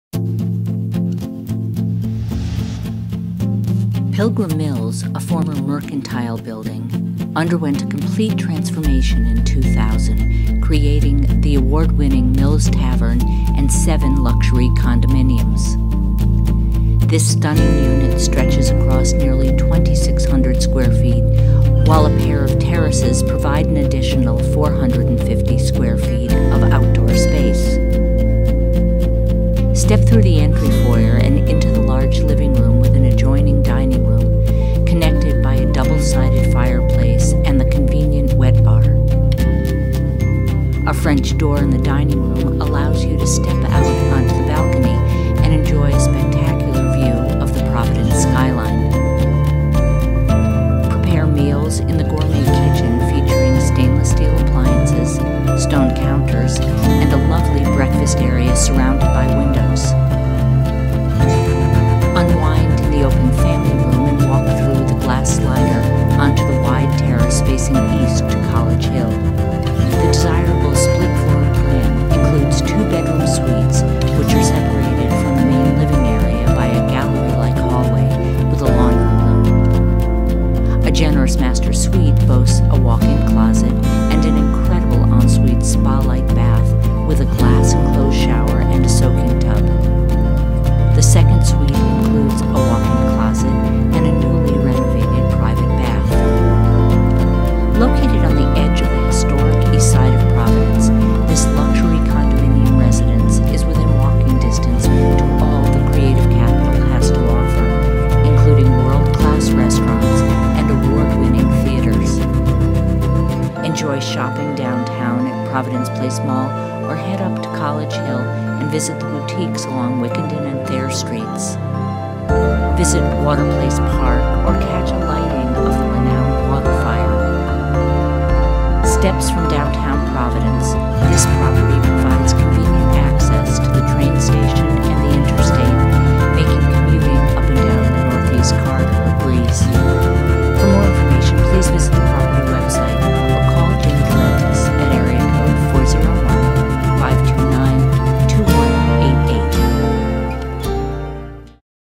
101 North Main Street Unit #600, East Side Of Providence, RI 02903
Pilgrim Mills, a former mercantile building, underwent a complete transformation in 2000, creating the award-winning Mills Tavern and seven luxury condominiums. This stunning unit stretches across nearly 2,600 square feet while a pair of terraces provide